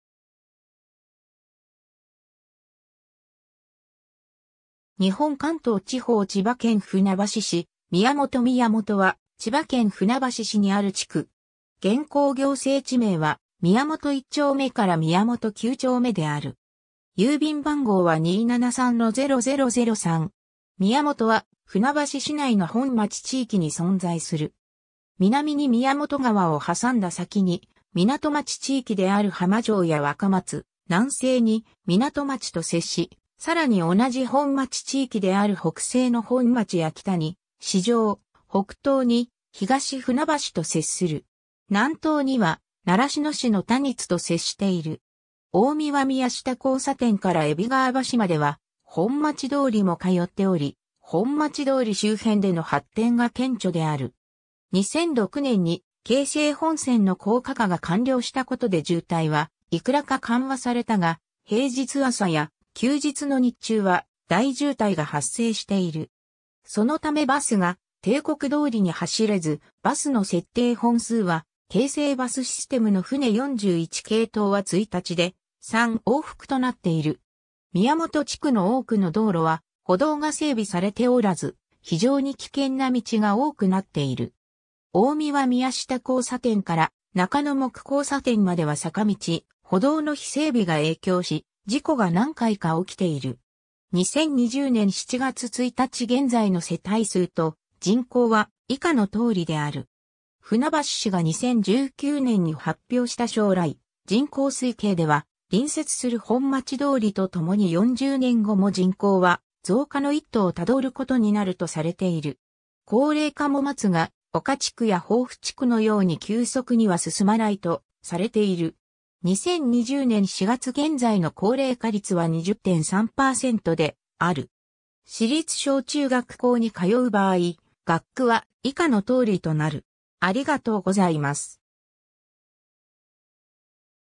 宮本 (船橋市)
船橋市の町・字
日本 」 関東地方 」 千葉県 」 船橋市 」 宮本 (船橋市) 宮本（みやもと）は、千葉県船橋市にある地区。
現行行政地名は宮本一丁目から宮本九丁目である。
 宮本は船橋市内の本町地域に存在する。
南に宮本川を挟んだ先に湊町地域である浜町や若松、南西に湊町と接し、さらに同じ本町地域である北西の本町や北に市場、北東に東船橋と接する。
南東には習志野市の谷津と接している。
 大神宮下交差点から海老川橋までは本町通りも通っており、本町通り周辺での発展が顕著である。
2006年に京成本線の高架化が完了したことで渋滞はいくらか緩和されたが、平日朝や休日の日中は大渋滞が発生している。
そのためバスが定刻通りに走れず、バスの設定本数は京成バスシステムの船41系統は1日で3往復となっている。
 宮本地区の多くの道路は歩道が整備されておらず、非常に危険な道が多くなっている。
大神宮下交差点から中野木交差点までは坂道・歩道の非整備が影響し、事故が何回か起きている。
 2020年（令和2年）7月1日現在の世帯数と人口は以下の通りである。
 船橋市が2019年（令和元年）に発表した将来人口推計では、隣接する本町通りと共に40年後も人口は増加の一途を辿ることになるとされている。
 高齢化も松が丘地区や豊富地区のように急速には進まないとされている。
2020年（令和2年）4月現在の高齢化率は20.3％である。